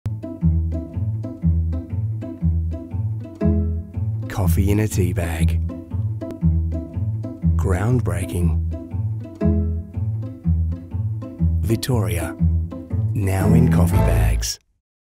Coffee in a tea bag. Groundbreaking.
A convenient, single serve coffee bag with 100% arabica coffee.

Vittoria, now in Coffee Bags.